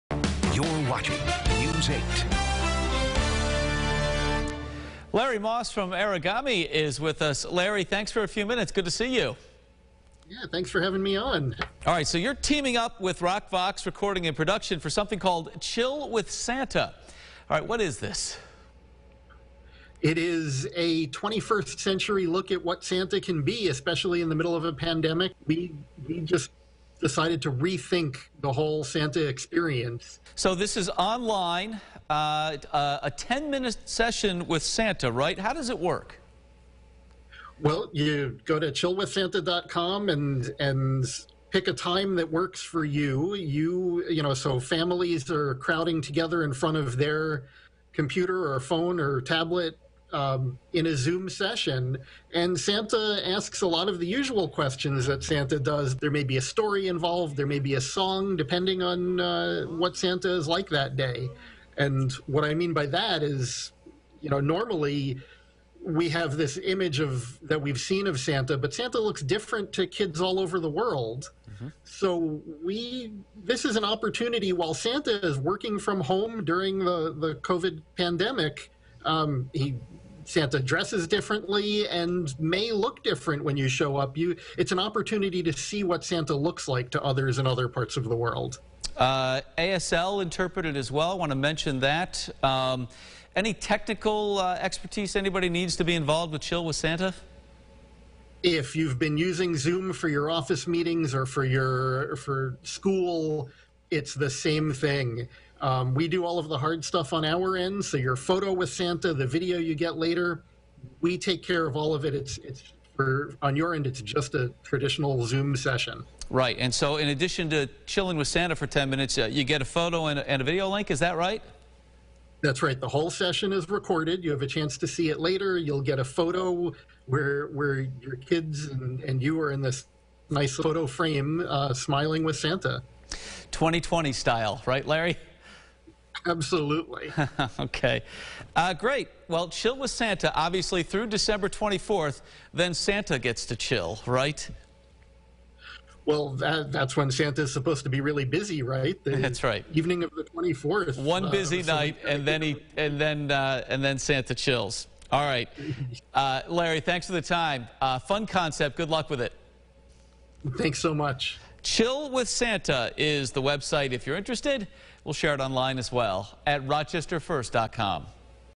Your chance to Chill with Santa!
In this age of COVID-19 families are being given an alternative to the traditional method of visiting with Santa before Christmas.